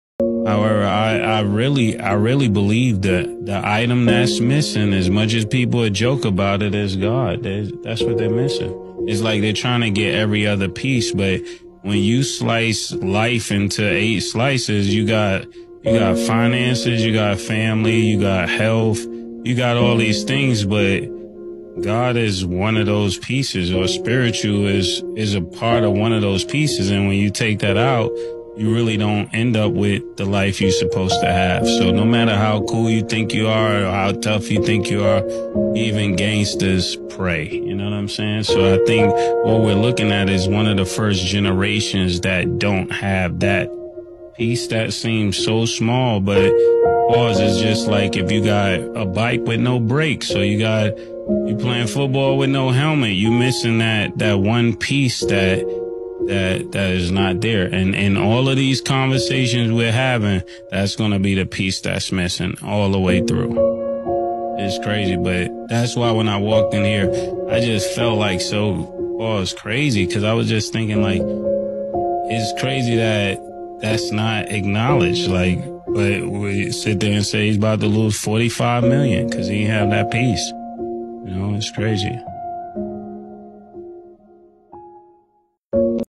Mase God is the missing piece from our lives
Psalm 127:1 NLT — A song for pilgrims ascending to Jerusalem. A psalm of Solomon.

Unless the LORD builds a house,
the work of the builders is wasted.
Unless the LORD protects a city,
guarding it with sentries will do no good.